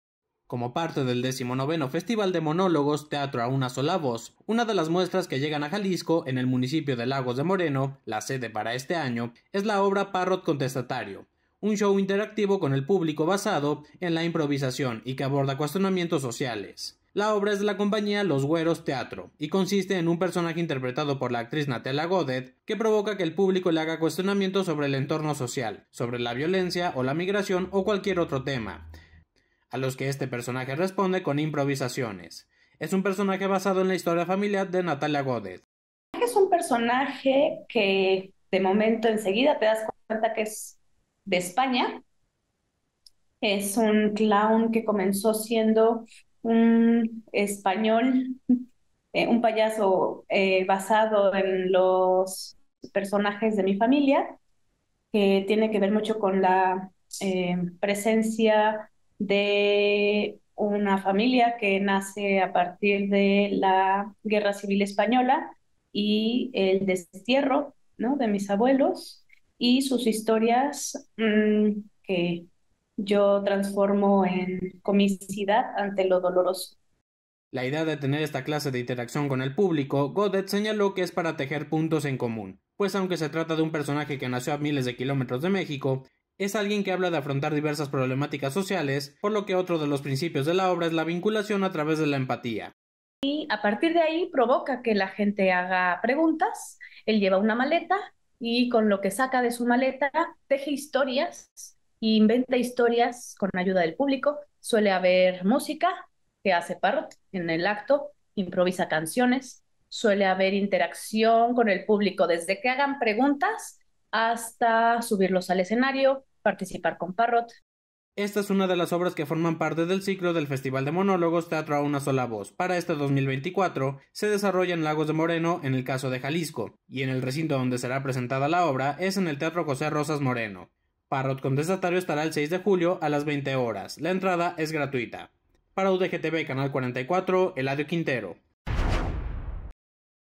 Festival de monólogos: La obra ‘Parrot Contestatario’ será presentada en Lagos de Moreno
Como parte del 19° Festival de Monólogos: Teatro a Una Sola Voz, una de las muestras que llegan a Jalisco en el municipio de Lagos de Moreno (la sede para este año), es la obra ‘Parrot contestatario’, un show interactivo con el público basado en la improvisación, y que aborda cuestionamientos sociales.